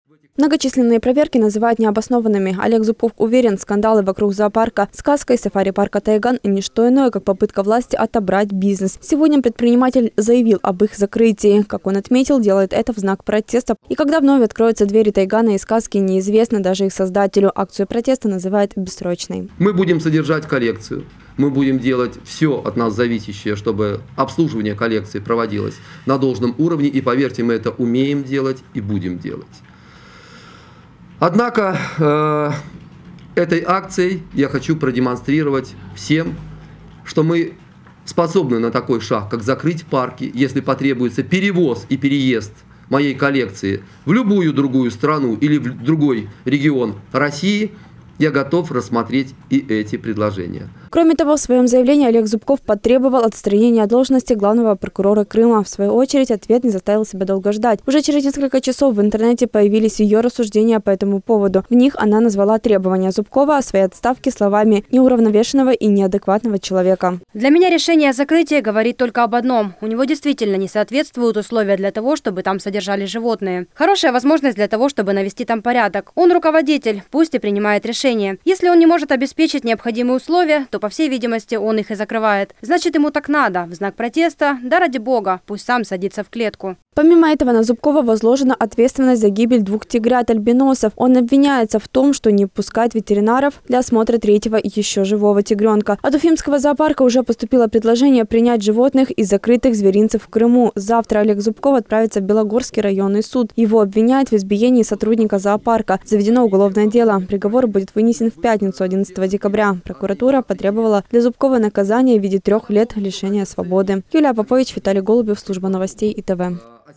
Сюжет – «Сказка» и «Тайган» закрылись
Ялтинский зоопарк «Сказка» и сафари-парк «Тайган» сегодня закрылись. Эту новость сообщил журналистам их владелец и директор Олег Зубков. Такое решение предприниматель принял самостоятельно. Как он отметил, не стал дожидаться, пока их закроют решением вышестоящих органов.